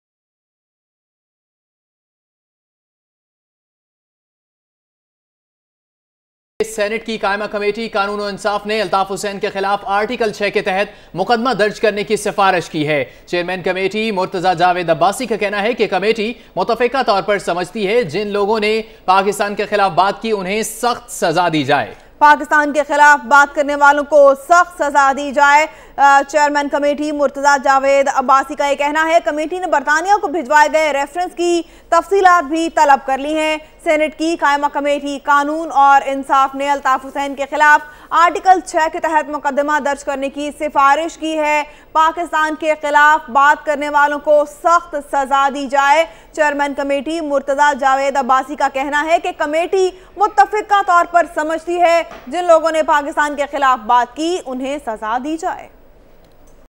24 Breaking: Standing Committee recommended prosecution under Article 6 against Altaf Hussain
24 Breaking: Standing Committee recommended prosecution under Article 6 against Altaf Hussain

24 News HD is one of the leading news channels of Pakistan bringing you the latest current affairs from Pakistan and around the world.